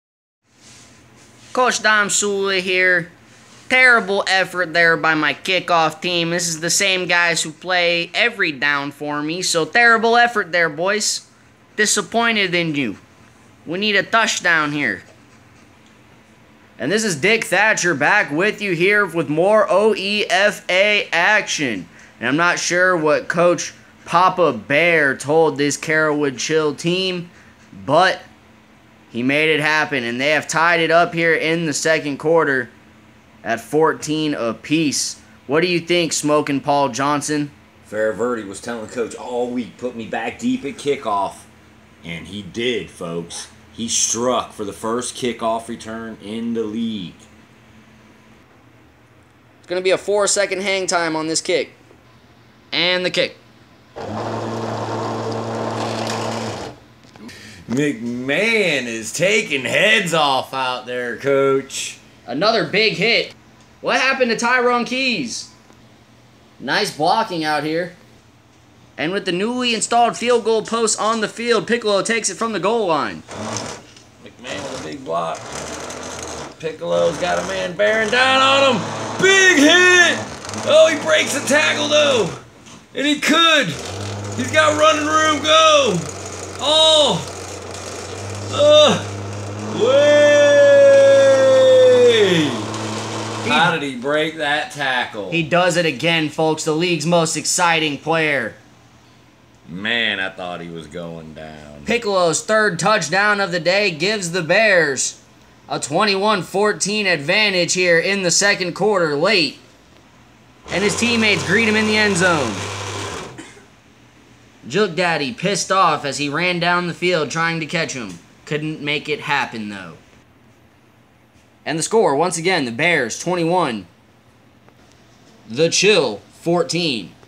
OEFA Regular Season Game 1 Bears Vs Chill Part 14
Another kickoff return touchdown!!!
21-14 Bears!! Piccolo with his 3rd score of the day